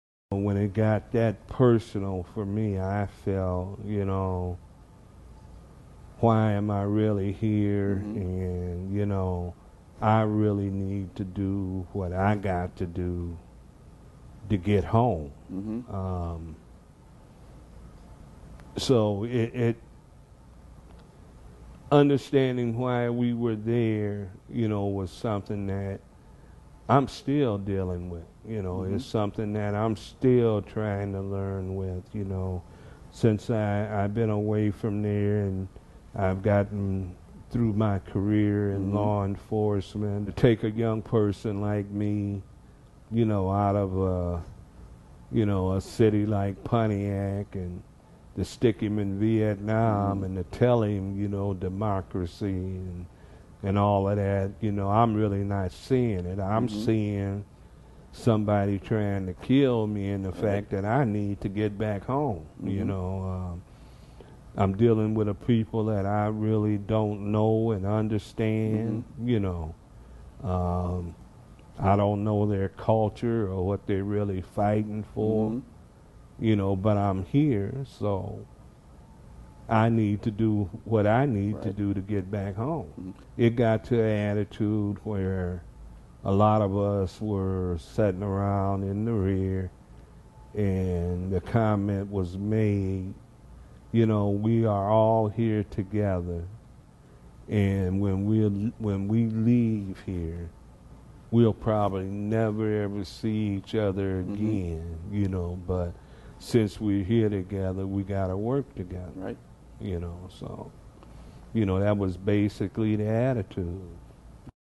Al Walker - Why Were We There
Interview from Grand Valley State University Veterans History Project